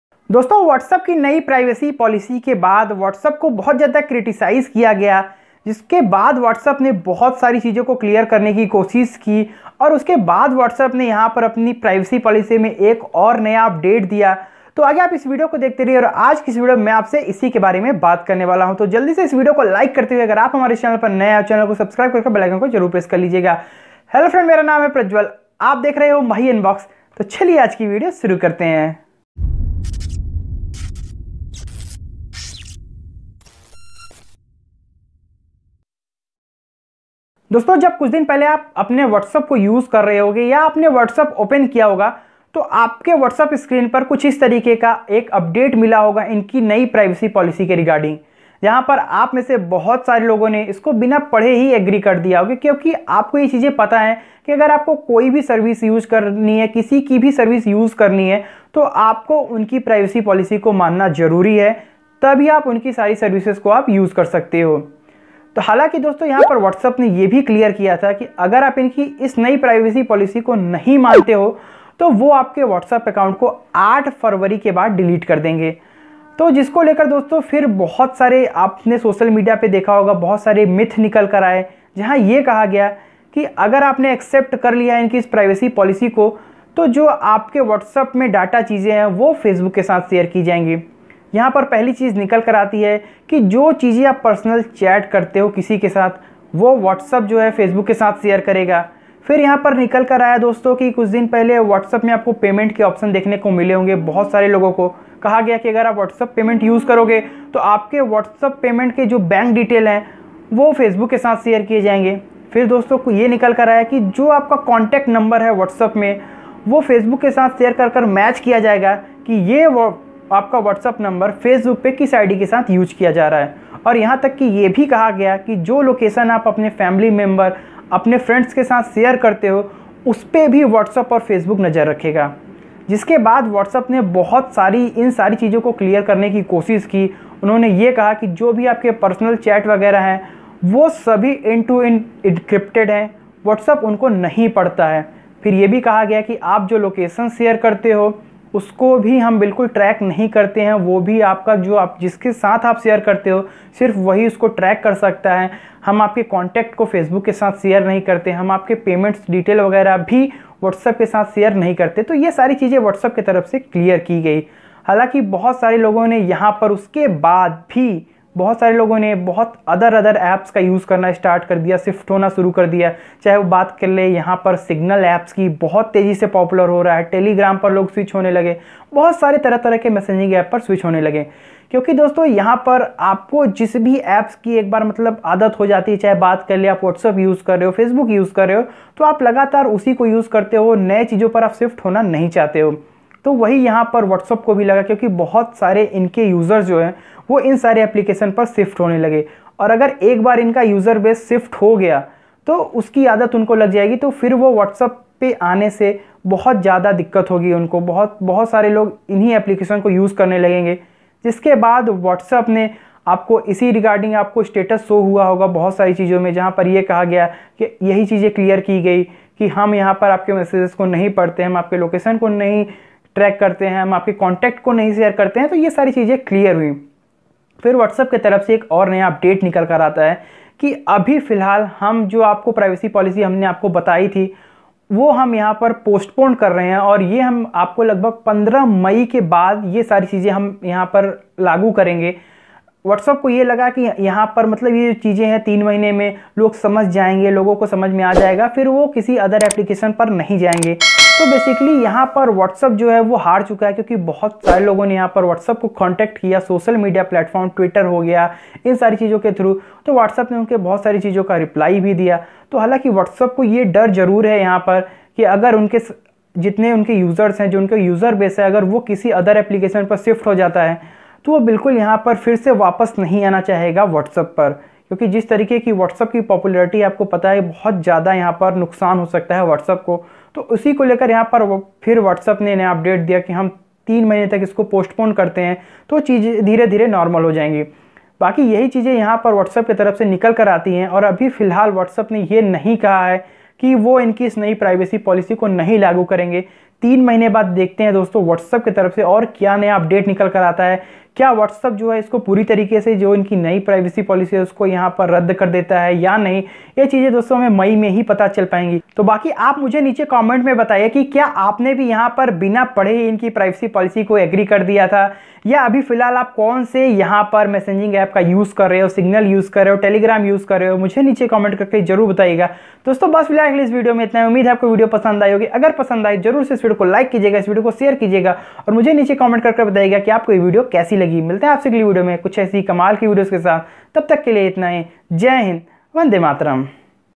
Whatsapp New Privacy Policy Update | We Won!🔥🔥🔥
Namaskar Dosto,aaj is video mein maine WhatsApp new Privacy policy update ke bare baat ki hai aur aap ko batya ki WhatsApp ki tarf se bahut sare chize zlear ki gayi hai. Mujhe umeed hai ye video aapko pasand aayega.